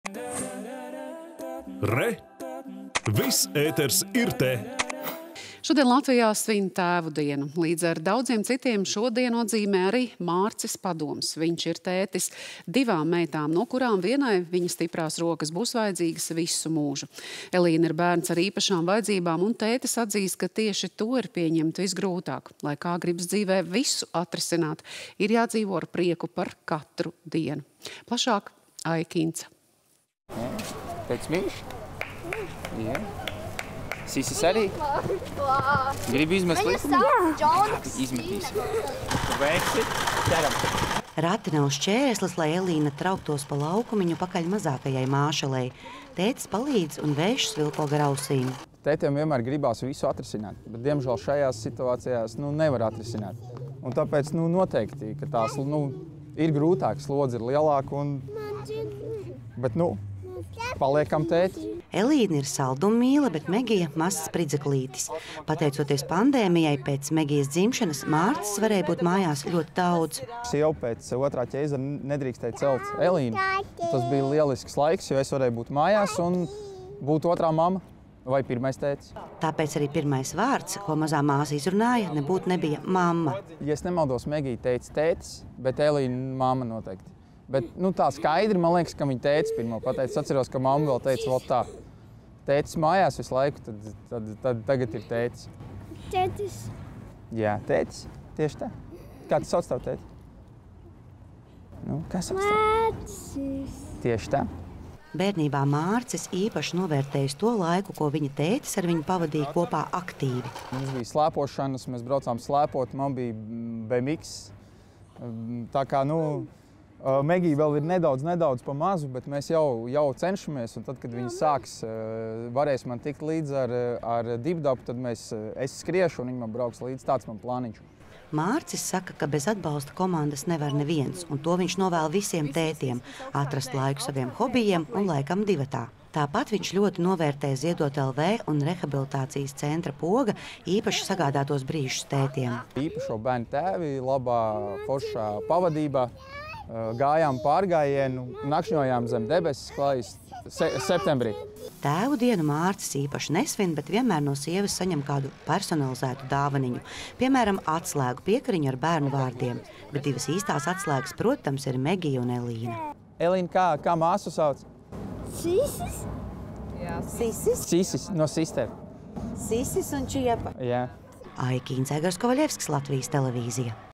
Divu meitu tētis: Novēlu tētiem atrast laiku arī sev
Šodien Latvijā svin Tēvu dienu. Līdz ar daudziem citiem šo dienu atzīmē arī Mārcis Padoms. Viņš ir tētis divām meitām, no kurām vienai viņa stiprās rokas būs vajadzīgas visu mūžu. Elīna ir bērns ar īpašām vajadzībām un tētis atzīst, ka tieši to vienmēr praktiskajam vīrietim ir pieņemt visgrūtāk. Lai kā gribas dzīvē visu atrisināt, ir lietas, kas vienkārši ir jāpieņem. Un jādzīvo ar prieku par katru dienu. Plašāk Aijas Kincas reportāžā.